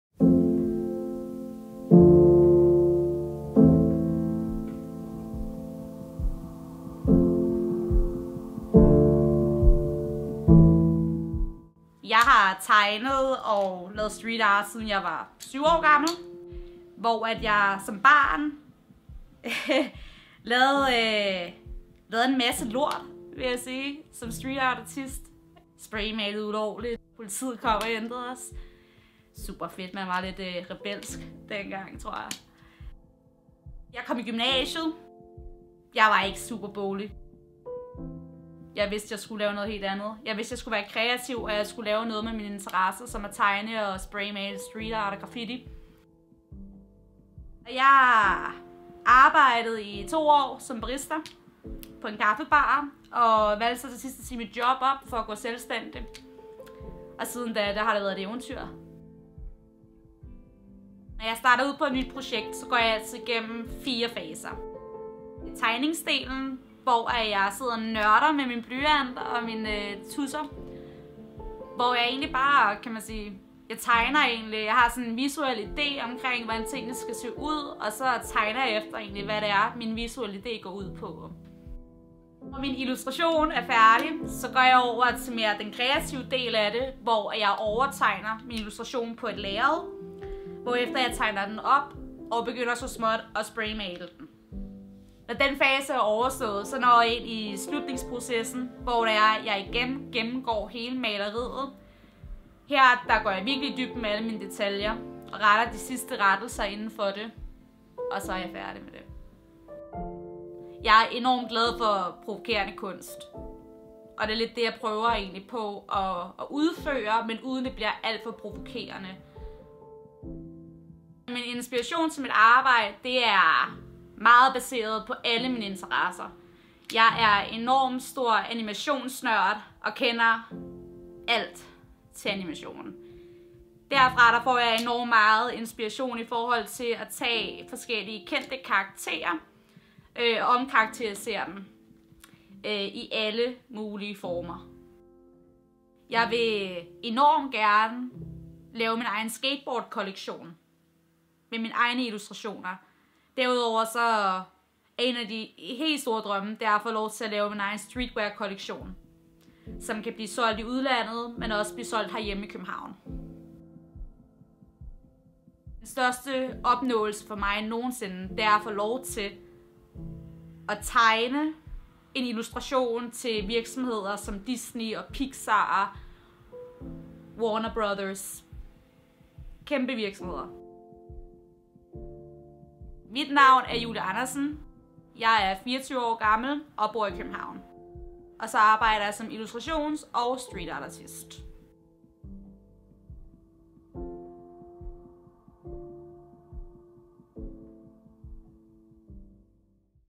Portrait of Julie L. Andersen (International street artist from Copenhagen)
Julie Andersen is a 24 year old illustration and street artist based in Copenhagen, Denmark.

I am Jonas Togo, a 25 year old Photographer and Filmmaker based in Copenhagen, Denmark. This is my journey 東郷